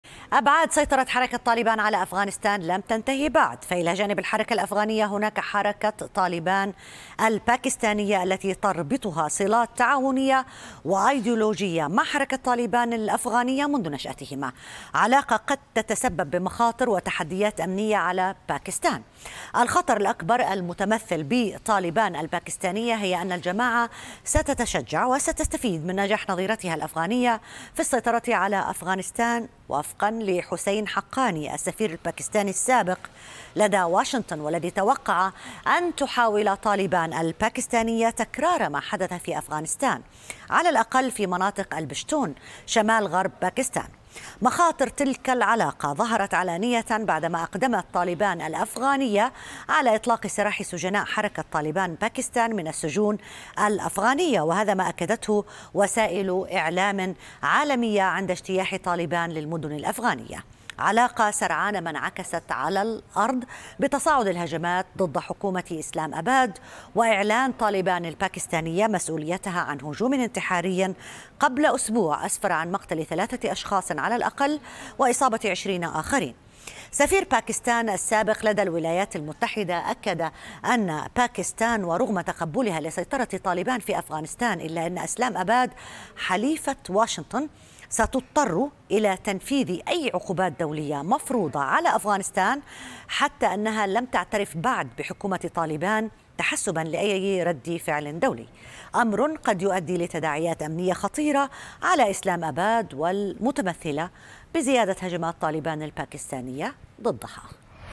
ماذا تعرف عن حركة طالبان الباكستانية؟
بعد سيطرة طالبان فرع أفغانستان على البلاد.. حركة طالبان الباكستانية التي تربطهما صلات تعاونية وإيدولوجية منذ نشأتهما تكشر عن أنيابها ضد إسلام أباد بمجموعة تفجيرات